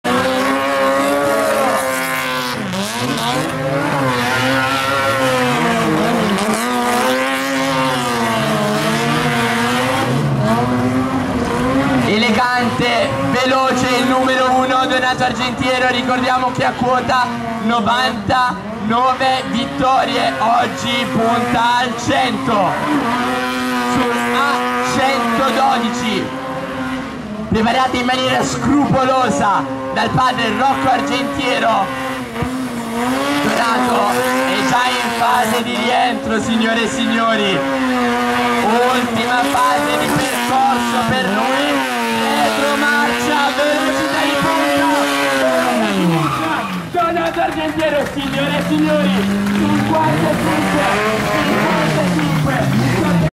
CGP2014 - Gimkana Santa Maria al bagno - Donato Argentiero 1° assoluto A112 (100^ vittoria)
Sono passati 11 anni, centinaia di giri veloci, tantissimi trofei ottenuti, ma soprattutto EMOZIONI offerte al pubblico, con una guida superlativa, pulita sempre veloce, anche estrema, insomma un vero CAMPIONE.
Molti avversari incontrati durante il cammino i quali lo hanno stimato e tutti sono onorati di poterlo sfidare nonostante la superiorità. Sempre umile e sportivo che ci tiene alle nuove generazioni ma guarda lui stesso con ammirazione ai suoi idoli, dal padre il grande Rocco Argentiero, il "Gigante Buono" delle gimkane, il suo avversario principale ma grande amico Matteo Marzio e molti altri.
Auguriamo a Donato di continuare a regalarci tanto spettacolo e le e forti emozioni di sempre!!!